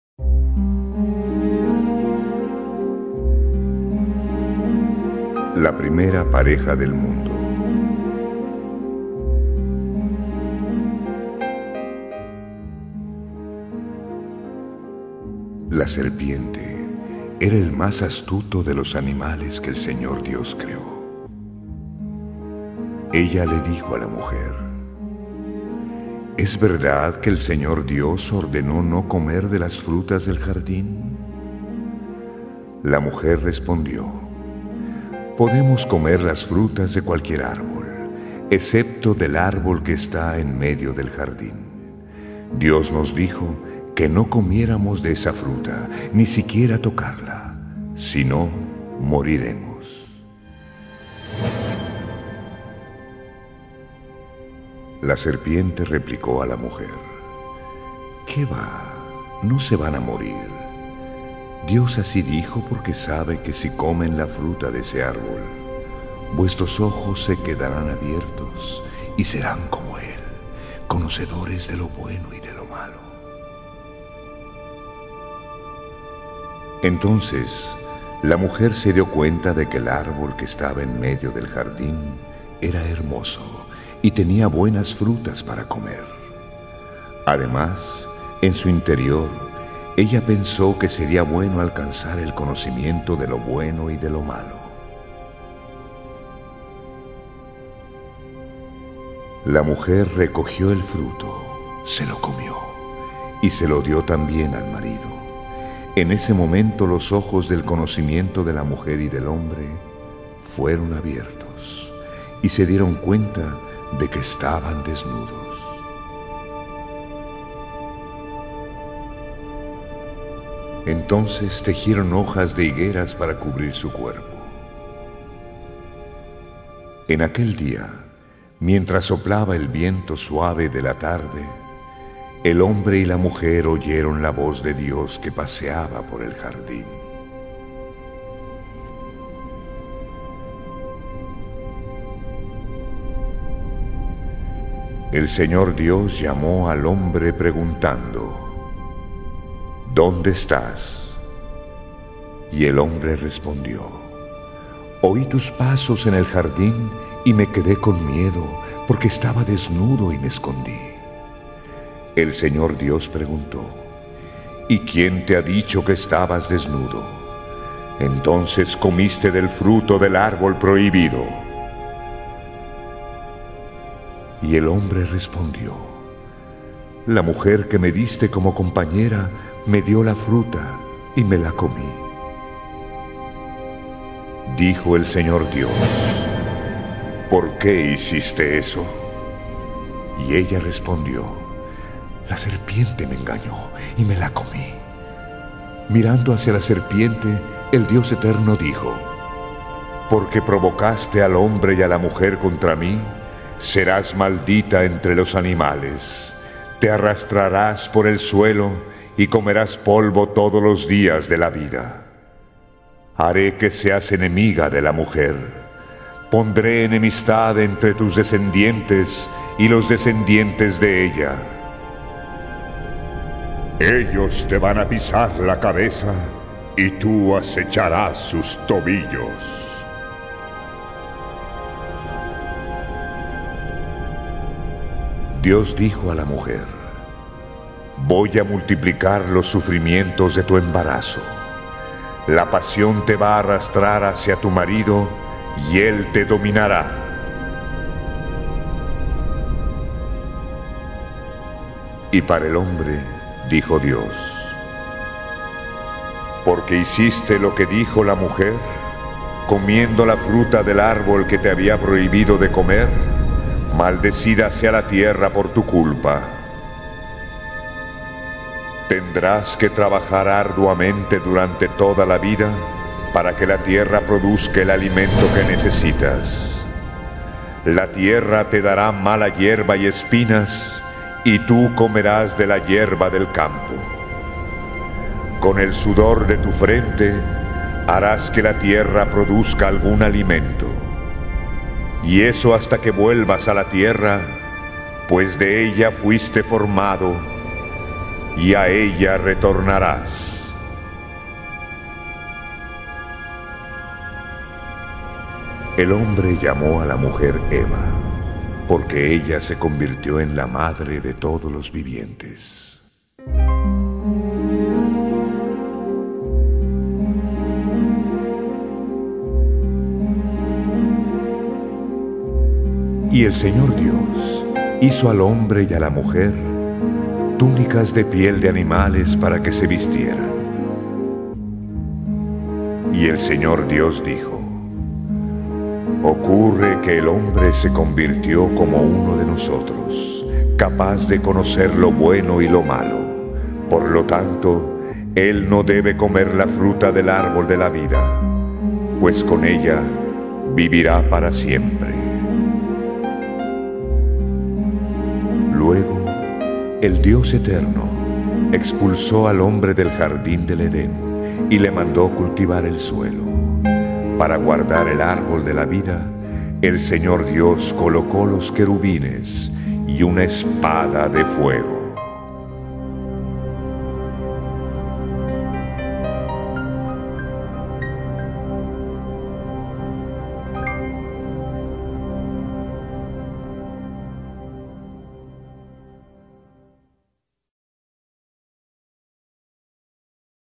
La Historia Sagrada Parte 3 La tentacion y caida
La Historia Sagrada Parte 3 La Tentación y Caída Génesis 3,1-24 Voz Enrique Rocha